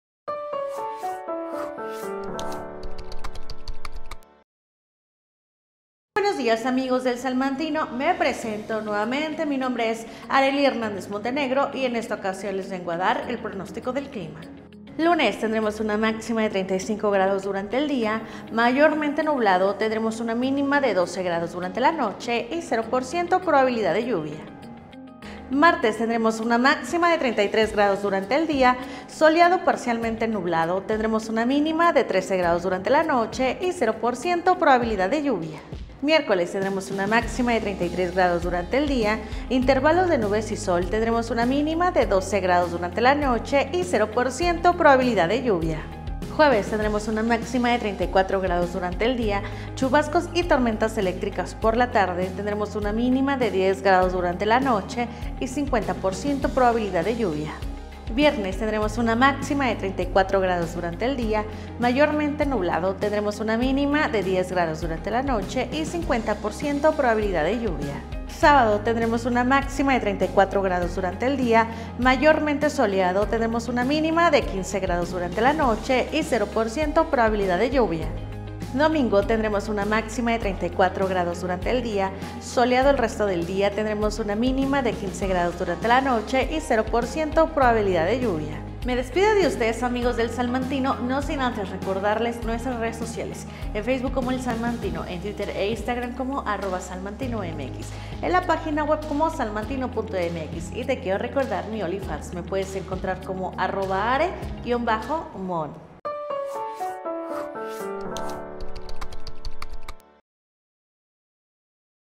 Clima del 29 de Abril al 5 de Mayo
BuenosDíasSalamanca Comienza la semana bien informado con nuestro pronóstico del clima, acompañados como ya es costumbre de Arely Hernández Montenegro.